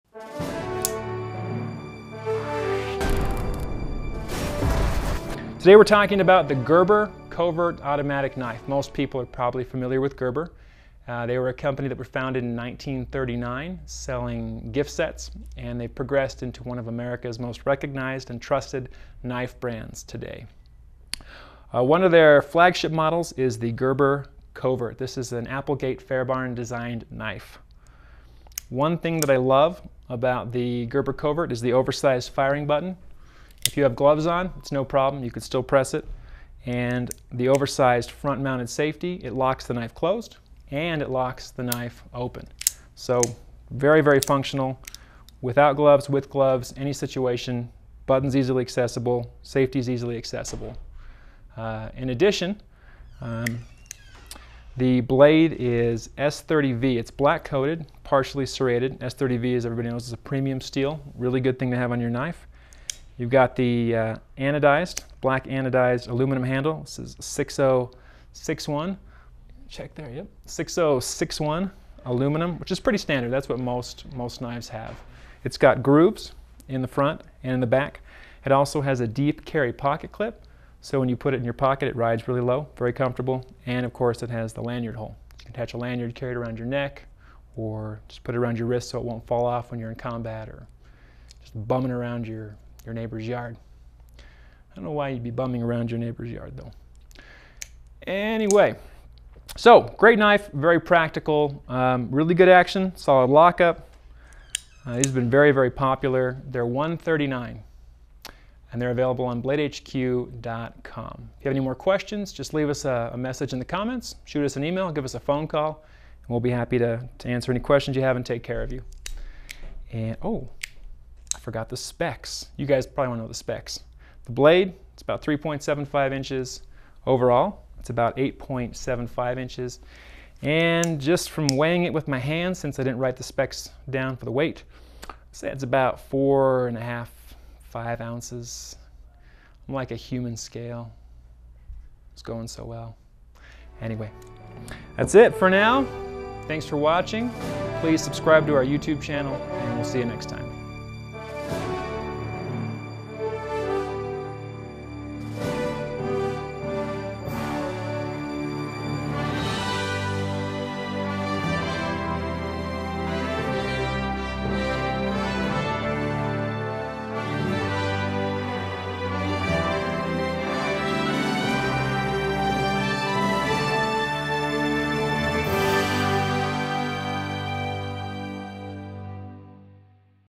Gerber Mini Covert Automatic Knife
The Gerber Mini Covert Automatic knife is an Applegate-Fairbairn design with a black finished S30V spear-point blade and a partially serrated edge. The Covert handle is black type III hard anodized aluminum with grip grooves. The satin firing button fires the blade open with great force and a front mounted slide safety locks the blade in both the open and closed positions. The removable deep carry tip-down pocket clip allows convenient and discreet carry. This comfortable and practical knife is perfectly sized for everyday carry. The Covert makes an excellent tactical knife choice.
Specifications:
Overall Length: 6.5"
Blade Length: 2.8"
Blade Material: S30V
Handle Material: Aluminum
Closed Length: 3.8"
Weight: 2.1 oz.